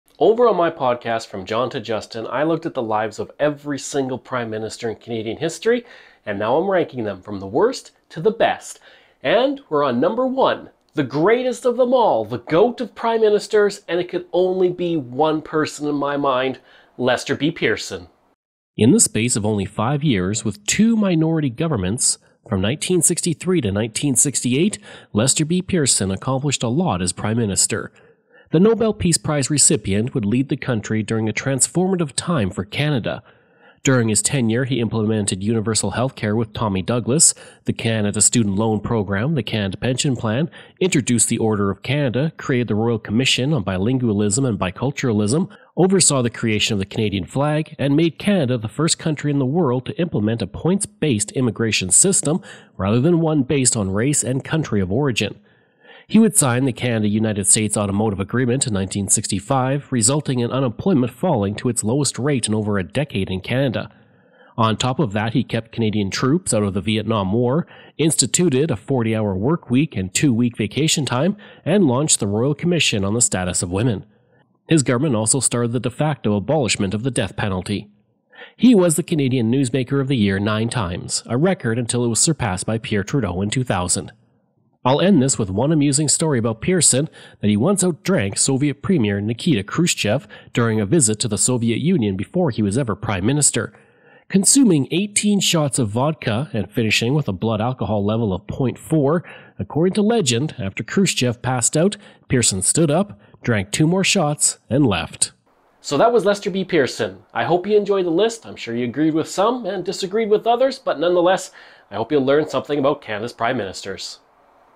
Ranking All 23 Canadian Prime Ministers: #1 - Lester B. Pearson
We have reached #1 of our 23 prime ministers, ranking them from worst to best: Lester B. Pearson

In the space of only five years, with two minority governments, Lester B. Pearson fundamentally altered Canada forever. He would introduce Universal Health Care, the Canada Pension Plan, Canada Student Loans, the Canadian flag, started the defacto abolishment of the death penalty and kept Canadian troops out of the Vietnam War.